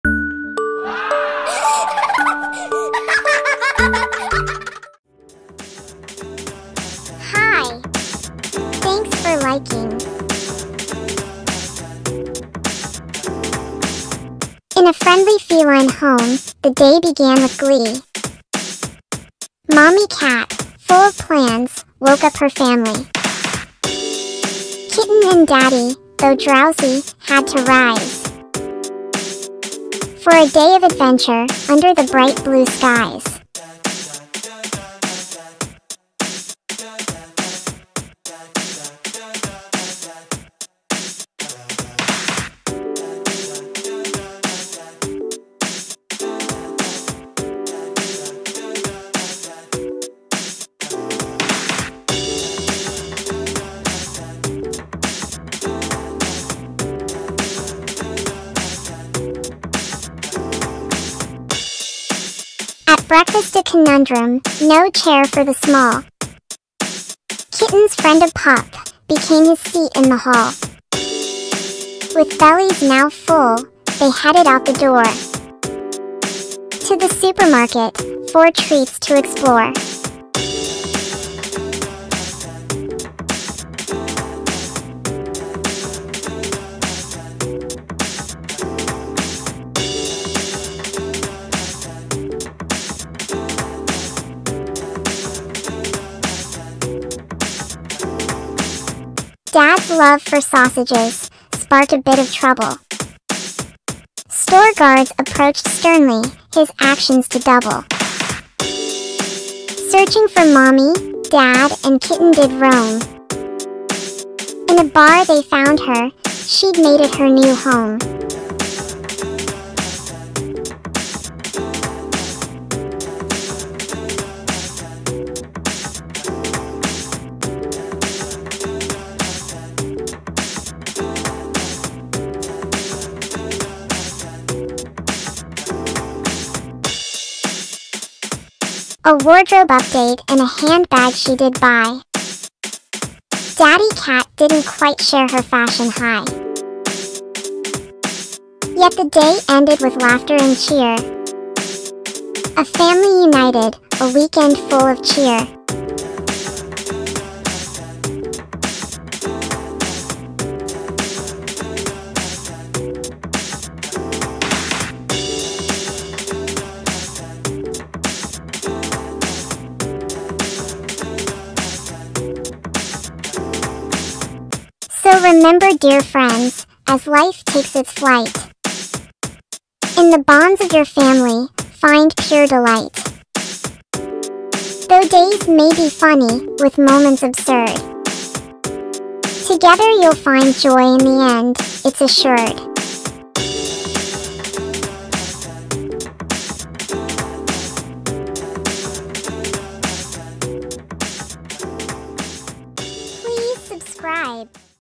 Cat Family's Shopping Spree😹 A Comical Adventure
❤️Join the adorable cat family on a hilarious day out filled with laughter and adventures! Mommy cat's day off takes an unexpected turn as she tries to get her sleepy family up and ready for a fun shopping trip. Watch as daddy cat, the kitten, and even a friendly puppy play their parts in this comical family escapade. From breakfast chair shenanigans to unexpected bar visits, this day is full of surprises!

In this funny cat family video, you'll see:

Funny breakfast moments with an inventive kitten
Dad's sausage-loving antics at the supermarket
Mom's shopping spree and a surprise bar visit
A heartwarming family adventure filled with laughter